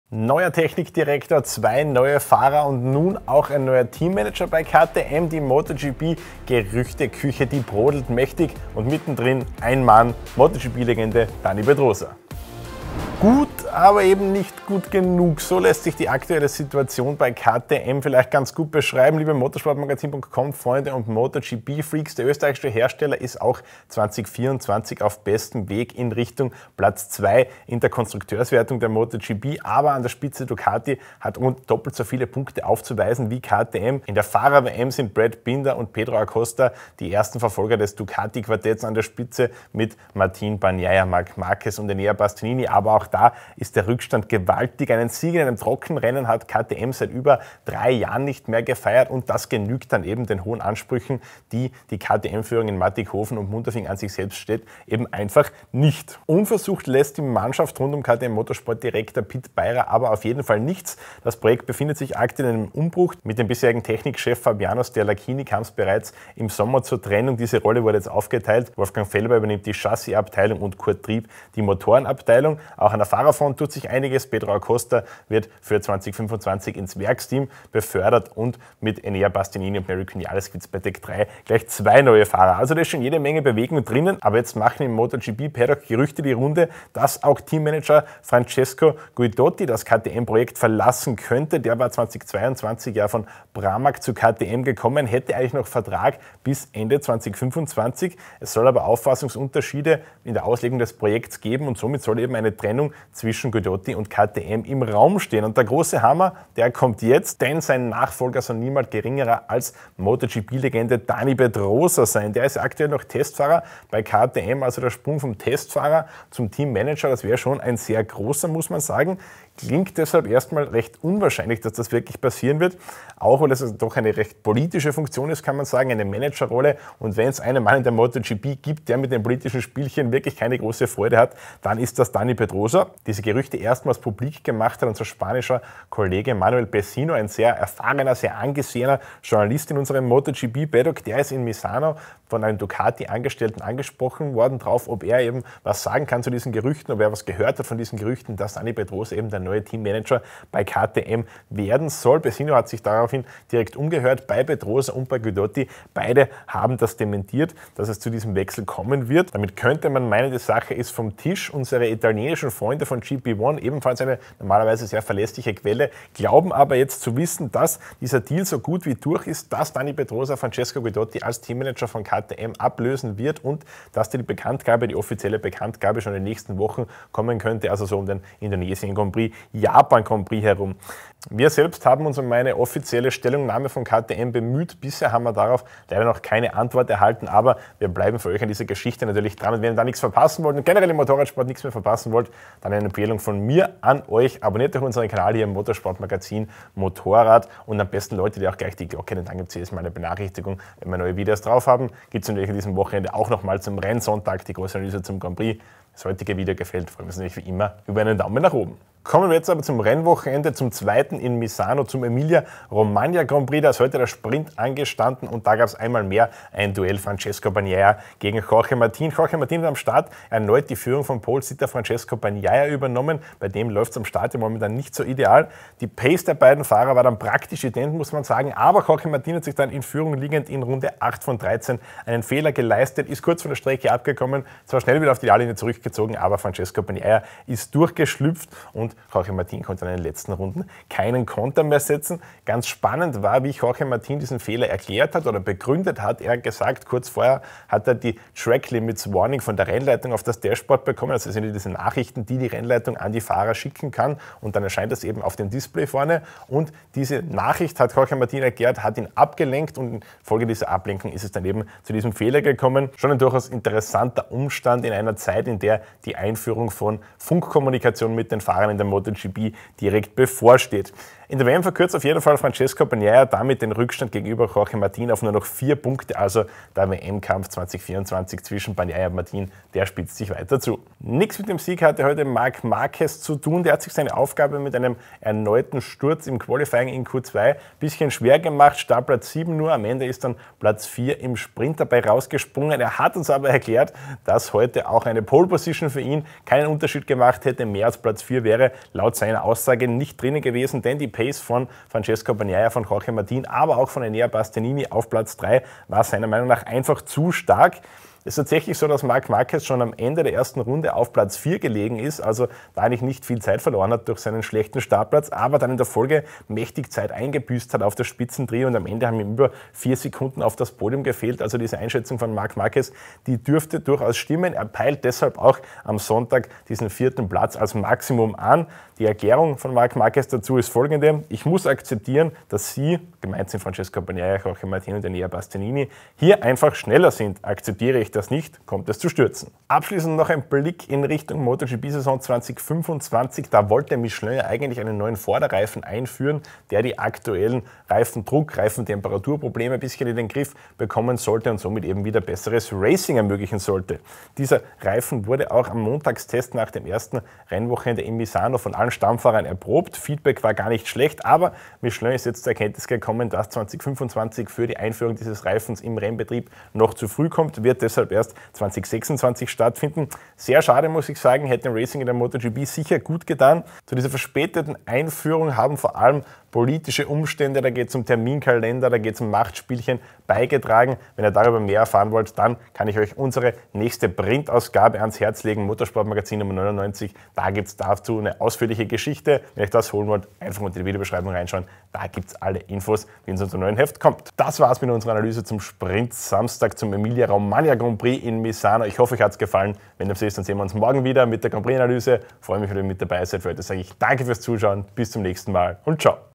MotoGP-Bombe! Dani Pedrosa bald neuer KTM-Boss?
Gut, aber eben nicht gut genug - so lässt sich die aktuelle Lage bei KTM zusammenzufassen. Die Österreicher sind 2024 erneut auf Kurs in Richtung Platz zwei in der MotoGP-Konstrukteurswertung. Ducati hat an der Spitze allerdings rund doppelt so viele Punkte vorzuweisen. Auch in der Fahrer-WM liegen Brad Binder und Pedro Acosta weit hinter dem führenden Ducati-Quartett zurück. Ein Sieg in einem MotoGP-Trockenrennen ist KTM seit mehr als drei Jahren nicht mehr gelungen. Das genügt den eigenen hohen Ansprüchen nicht - und so kam es in den vergangenen Monaten zu einer Reihe an Umstrukturierungen. Gerüchten zufolge soll eine weitere bevorstehen: Dani Pedrosa könnte demnach neuer Team-Manager werden. Wir ordnen die Spekulationen ein.

Wir suchen Verstärkung! Bewirb dich jetzt bei Motorsport-Magazin

Übersicht:
00:00 Intro & Themen
00:24 KTM: Pedrosa neuer starker Mann?
03:36 Bagnaia vs. Martin: Enges Duell in Misano
04:41 Marquez: Nicht schnell genug
05:42 Michelin: Kein neuer Reifen 2025